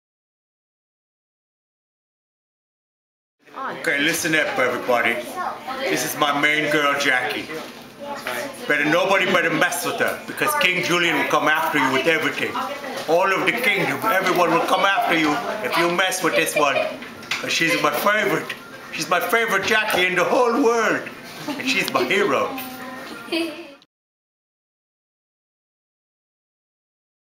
All Hail King Julien with Danny Jacobs and Jackie
Another fun day with the DreamWorks artists and special guest, Danny Jacobs, at Children's Hospital LA